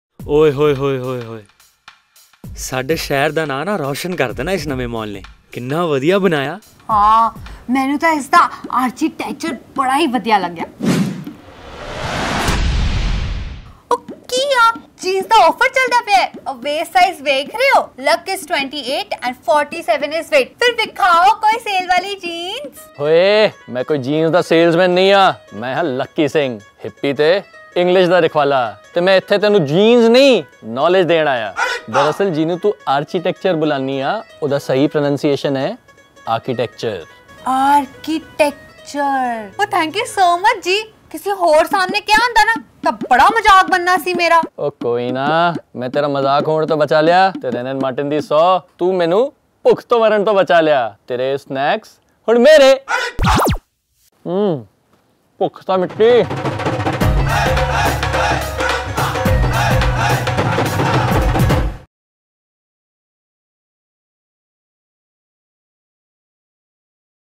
Prepositions ☑️ with Lucky Singh! | Hipi
How to pronounce 'Architecture' 🤔 Our superhero, Lucky Singh will clear the clouds of confusion daunting over your head in a zap! Watch the thrilling episode on 𝐇𝐢𝐩𝐢 to get your English pronunciations straight!